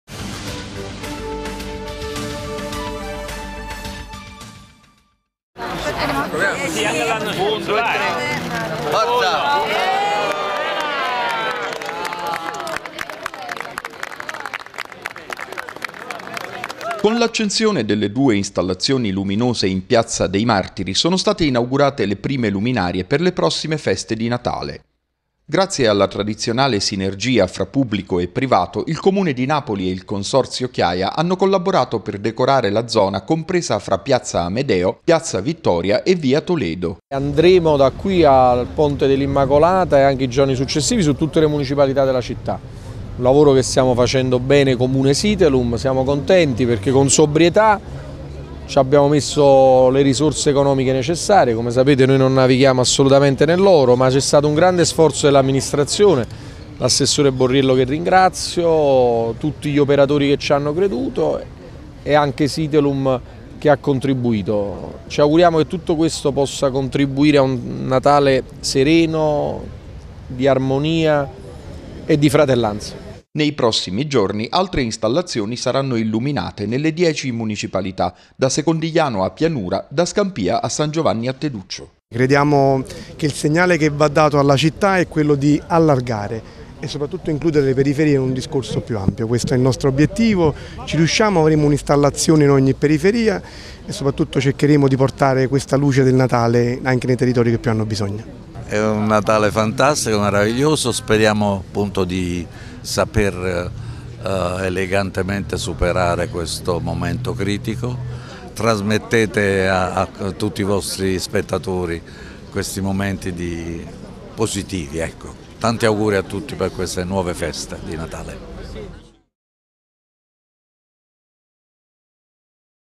La città s'illumina per le feste
Con l’accensione delle installazioni luminose in piazza dei Martiri sono state inaugurate le prime luminarie per le prossime feste di Natale; nei giorni seguenti saranno accese altre decorazioni in tutte le dieci municipalità.